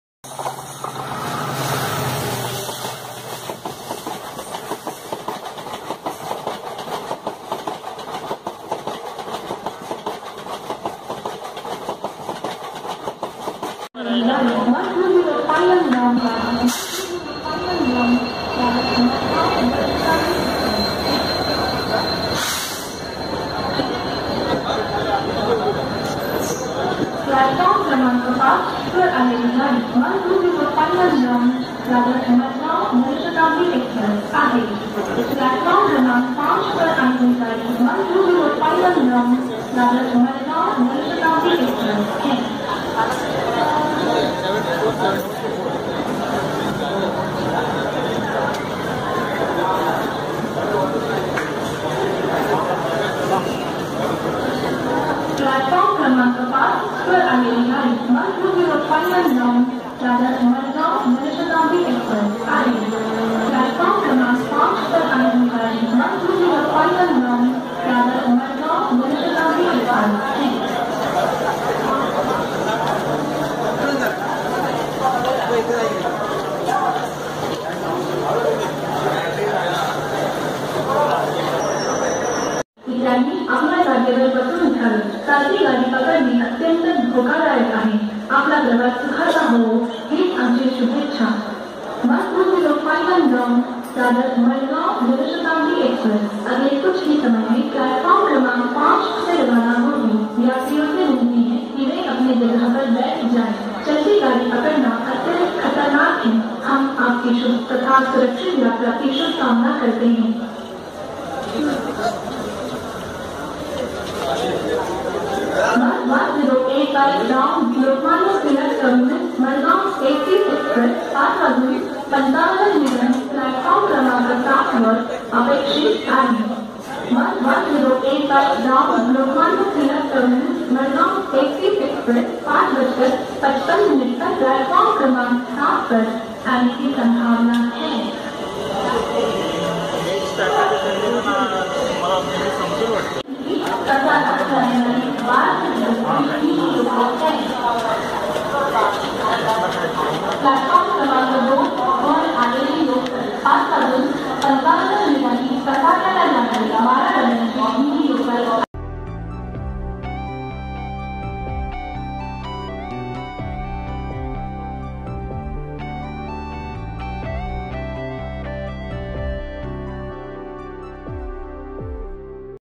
12051 Dadar - Madgaon Jan Shatabdi Express Arrives and Departs from Thane Railway Station
Date of Video: 2 Feb 2018.

12051 Dadar - Madgaon Jan Shatabdi Express Arrives and Departs from Thane Railway Station

Keep Watching !!

Video by,
Omkar Gawas.